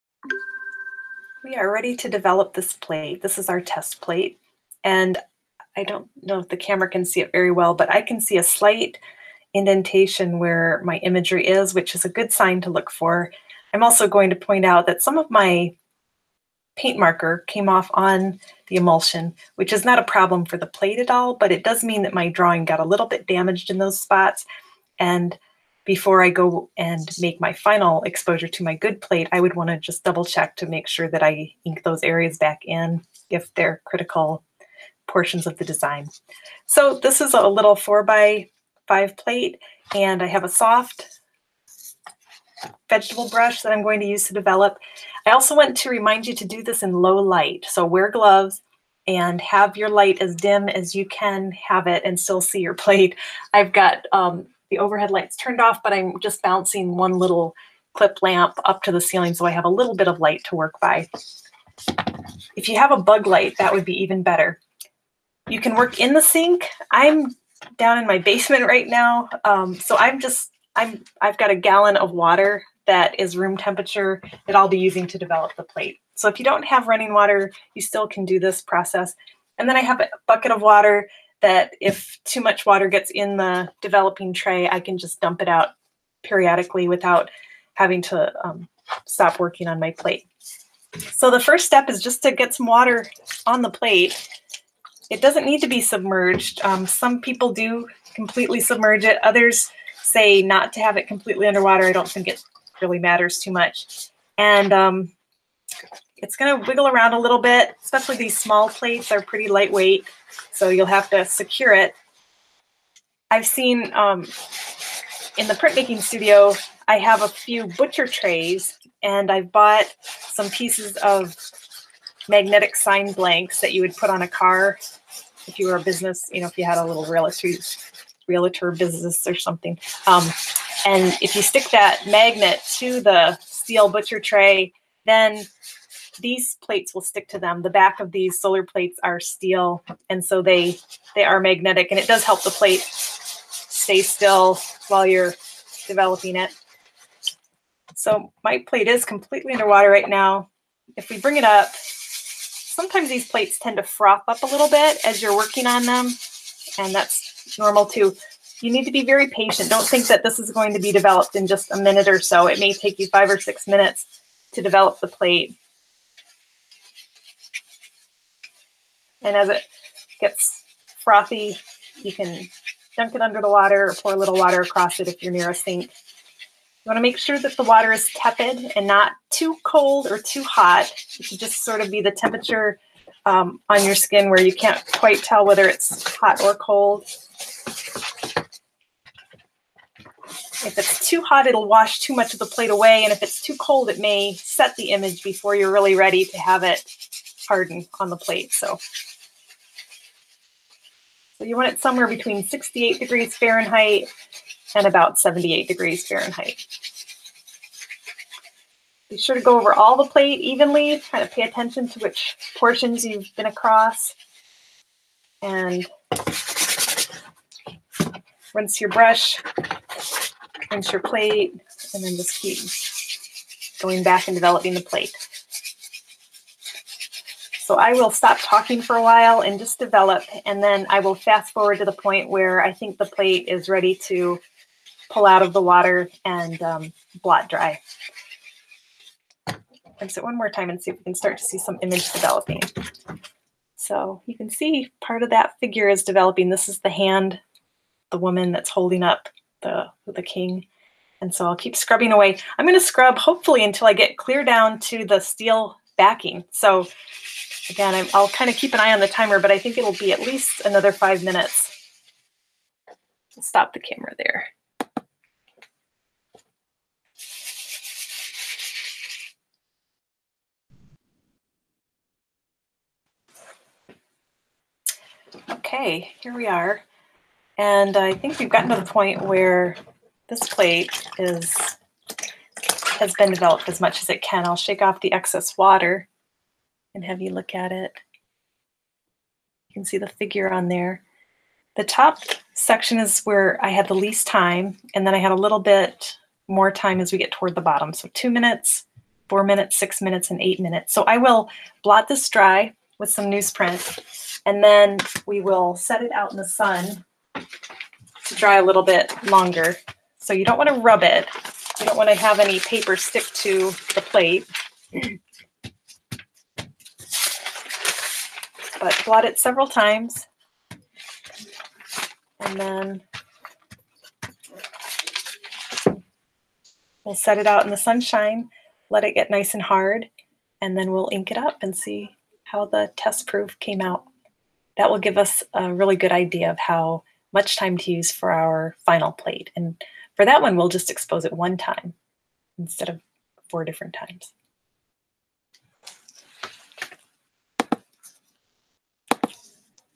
8e Developing the Step Test Exposure Solarplates-cc
This shows how to develop a Solarplate to be printed as a relief print